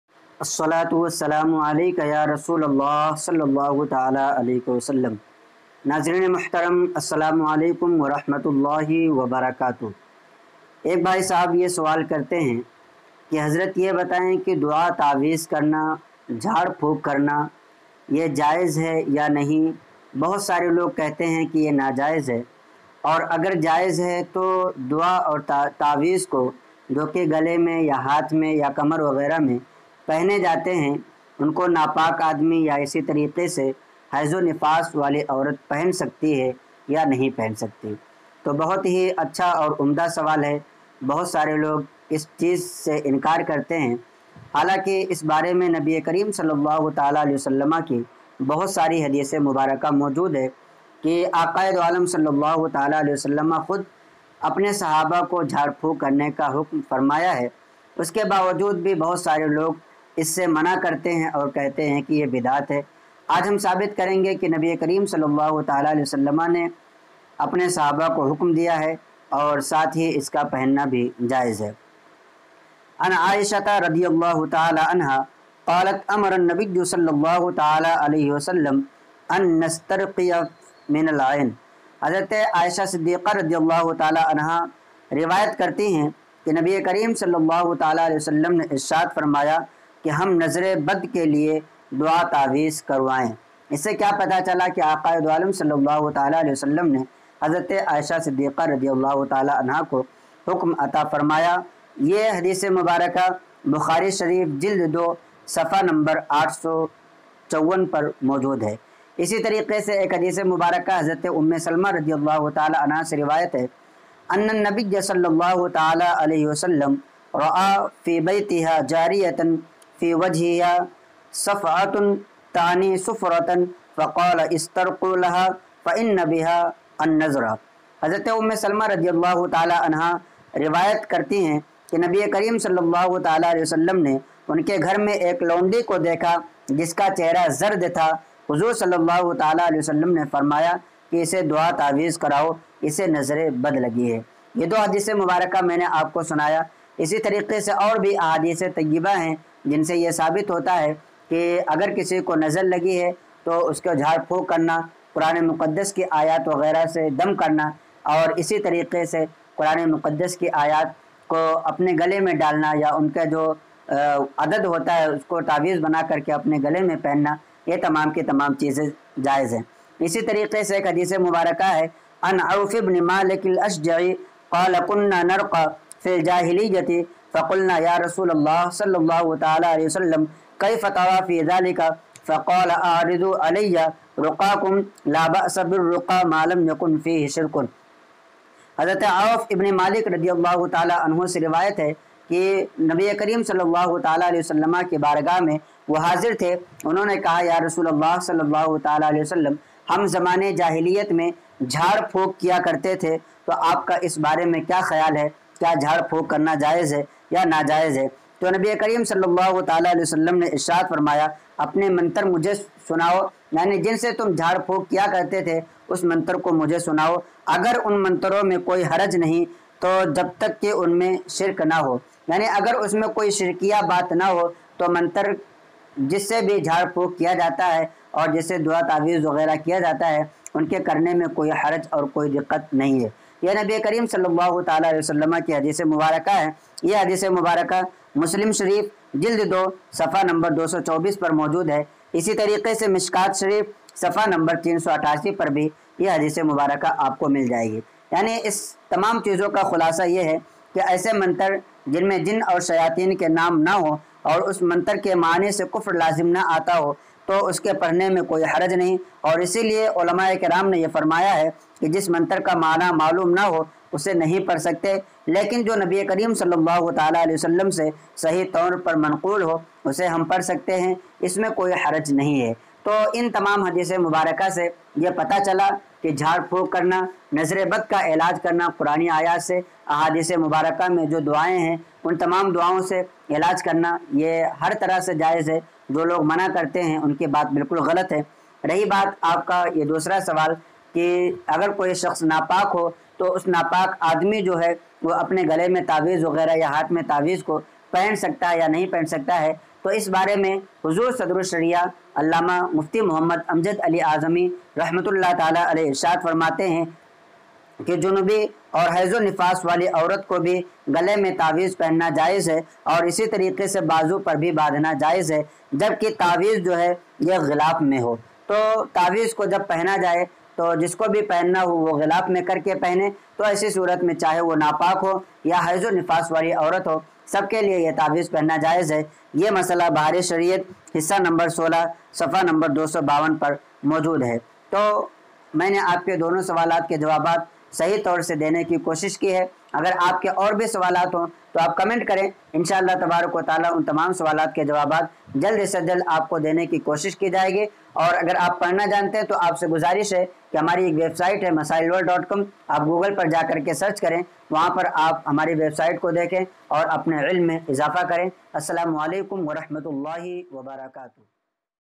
Jhad phook, aur dua, taweez haram hai! | Kya napak aurat taweez pahen sakti hai?
Deeni Masail Padhne Ke Liye Visit Karen